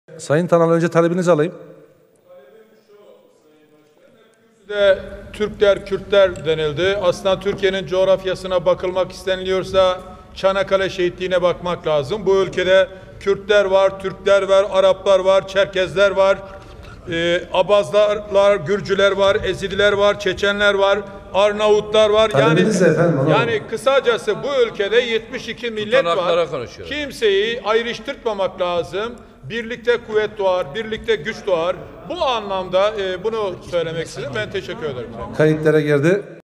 Genel Kurul 18 12 2017
CHP İstanbul Milletvekili Av.Mahmut TANAL'ın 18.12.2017 tarihinde Genel Kurul'da yaptığı konuşma